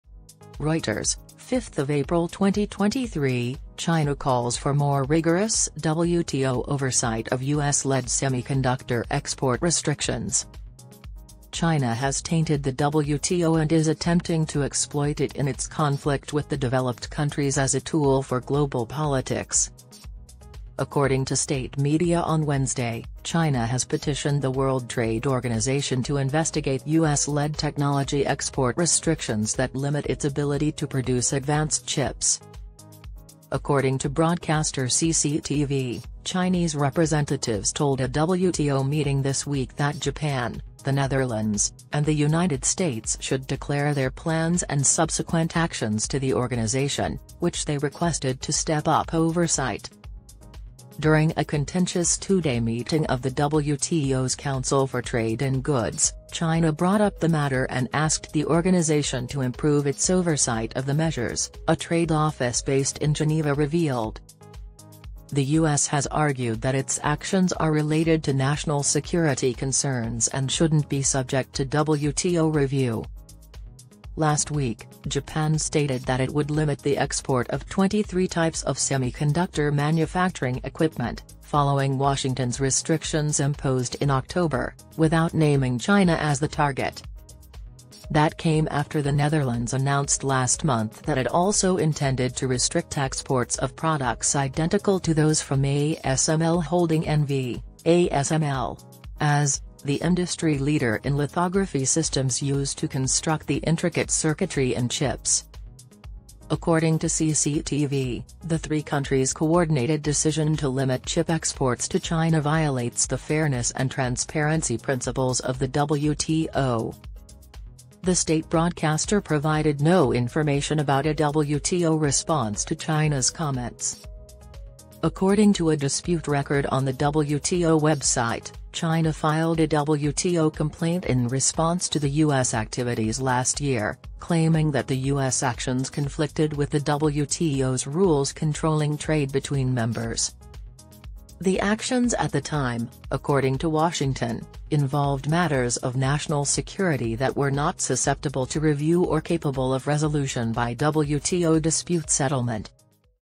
WTO Violator China calls for more rigorous WTO oversight of US-led semiconductor export restrictions
China calls for more rigorous WTO oversight of US-led semiconductor export restrictions.

China has tainted the WTO and is attempting to exploit it in its conflict with the developed countries as a tool for global politics.

According to state media on Wednesday, China has petitioned the World Trade Organization to investigate US-led technology export restrictions that are intended to limit its ability to produce advanced chips.

According to broadcaster CCTV, Chinese representatives told a WTO meeting this week that Japan, the Netherlands, and the United States should declare their plans and subsequent actions to the organization, which they requested to step up oversight on the topic.

During a contentious two-day meeting of the WTO's Council for Trade in Goods, China brought up the matter and asked the organization to improve its oversight of the measures, a trade official based in Geneva revealed.

The US has argued in the past that its actions are related to national security concerns and shouldn't be subject to WTO review.

Japan stated last week that it would limit the export of 23 different types of semiconductor manufacturing equipment, following Washington's restrictions imposed in October, without naming China as the target.

That came after the Netherlands announced last month that it also intended to restrict exports of products identical to those from ASML Holding NV (ASML.AS), the industry leader in lithography systems used to construct the intricate circuitry in chips.

According to CCTV, the three countries' coordinated decision to limit chip exports to China "violates the fairness and transparency principles of the WTO."

No information about a WTO response to China's comments was provided by the state broadcaster.

According to a dispute record posted on the WTO website, China filed a WTO complaint in response to the US activities last year, claiming that the US actions were in conflict with the WTO's rules controlling trade between members.

The actions at the time, according to Washington, involved matters of national security that were "not susceptible to review or capable of resolution" by WTO dispute settlement.